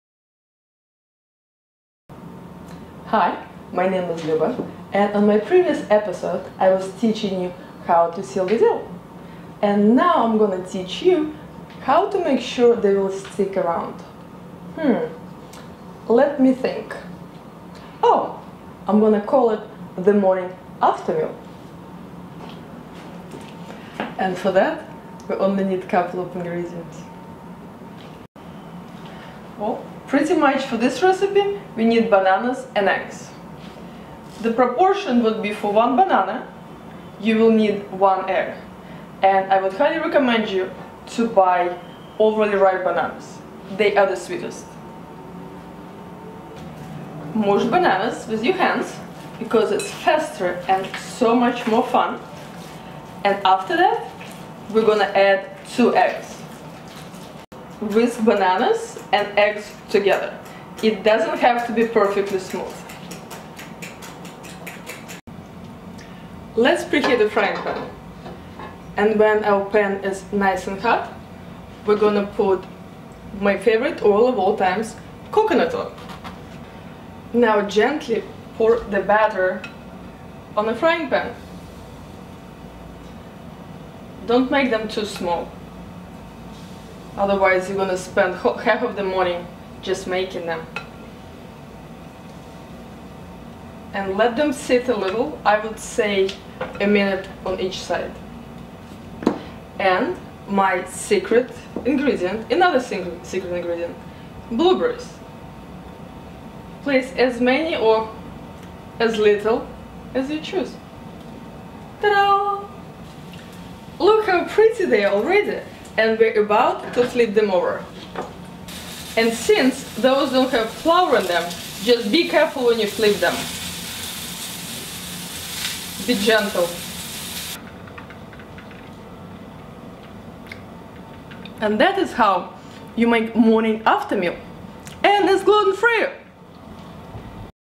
Luba's Morning After Meal Gluten Free Banana Pancakes
Banana pancakes , gluten free fun option to impress someone ! Have you ever made simple gluten free banana pancakes with blueberries to die for ?